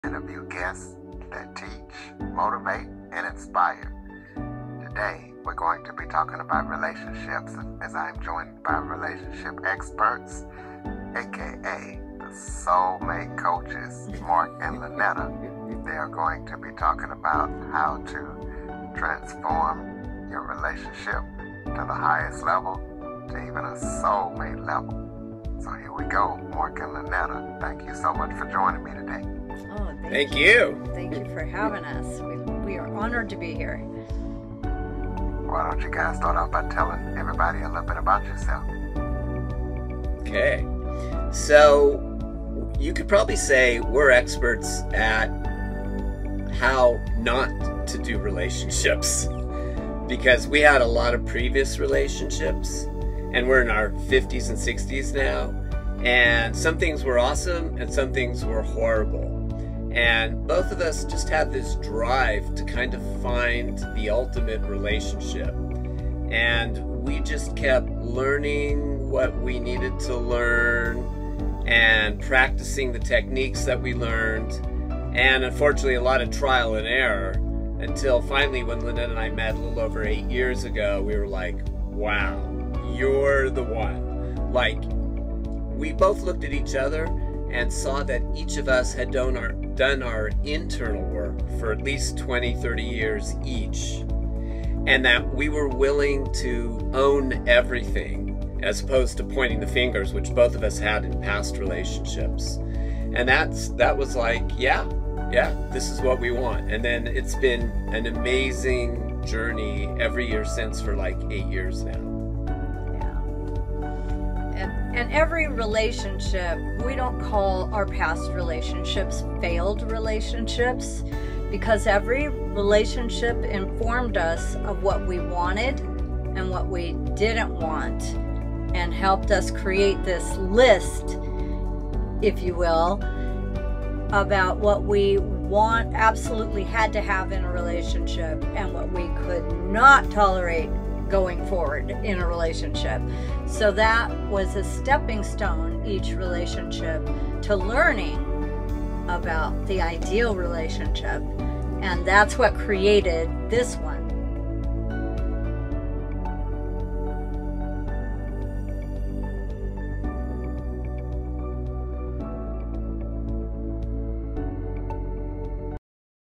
Every Relationship is a Stepping Stone
Mark and Lynetta  don’t see past relationships as failures but rather stepping stones to creating what we really want in relationship!

Your past relationships help you to create your list of what you want and what you don’t want going forward.

Books:
Three Doors to the Soul by Mark Siedler

Do the Work by Lynetta Avery

Do the Work Mobile App (includes written, audio and video versions)